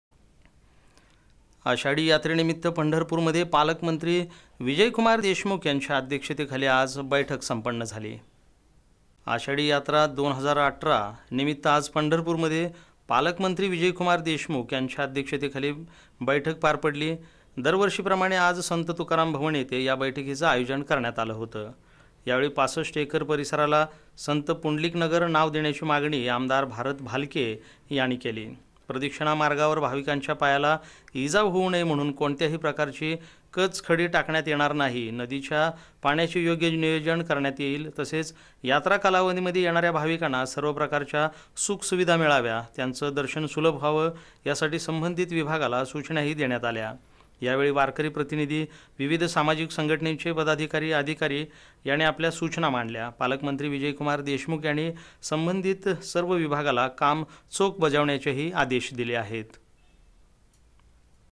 आषाढी यात्रेनिमित्त पंढरपूरमध्ये पालकमंत्री विजयकुमार देशमुख यांच्या अध्यक्षतेखाली
आषाढी यात्रेनिमित्त पंढरपूरमध्ये पालकमंत्री विजयकुमार देशमुख यांच्या अध्यक्षतेखाली बैठक संपन्न.@SBN मराठी.